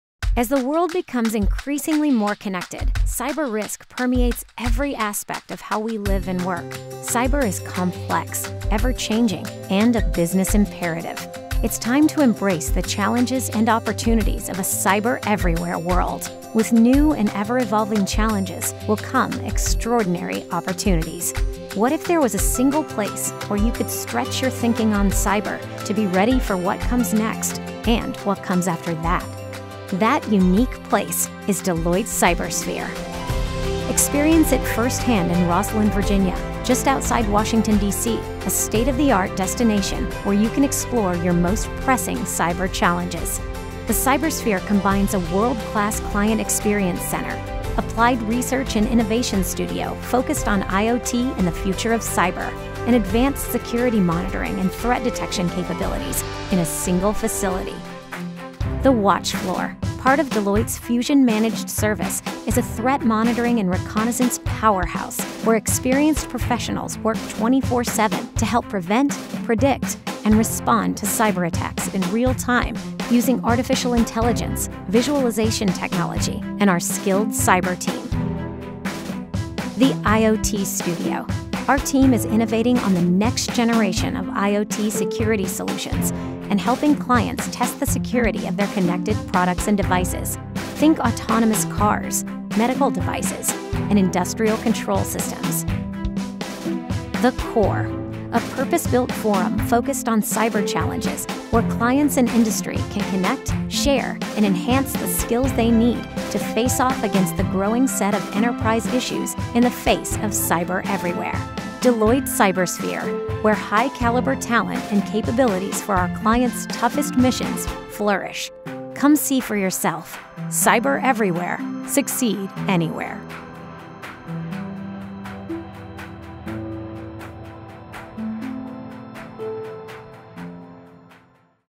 Cybersphere
Welcome to the Cybersphere. Deloitte’s state-of-the-art 25,000+ square foot destination for exploring your most pressing cyber challenges, for creating and testing the security of tomorrow’s Internet of Things (IoT) innovations, and for gathering to learn, share, and define a path forward for your organization, employees, customers, and beyond.